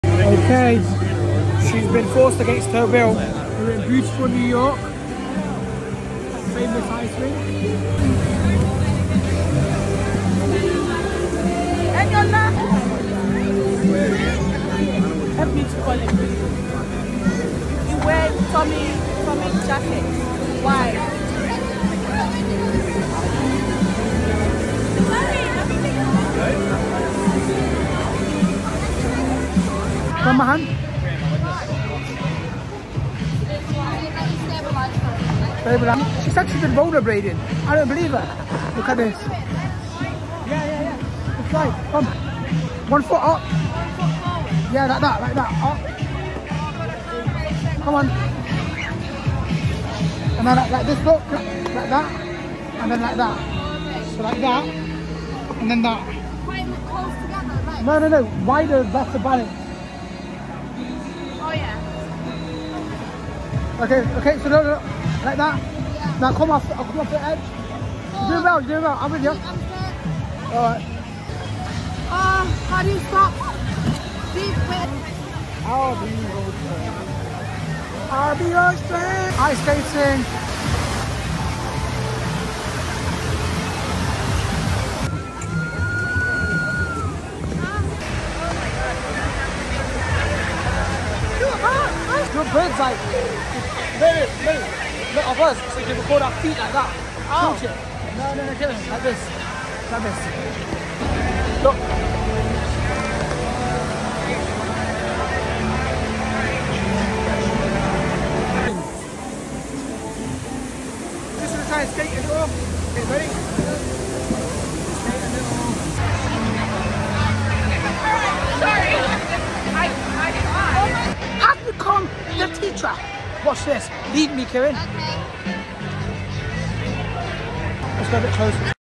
Skating in Newyork at The Rink at Rockefeller Centre
Ended up with me giving Kiran her first ice skating lesson, I’m defo like a pushy parent 😂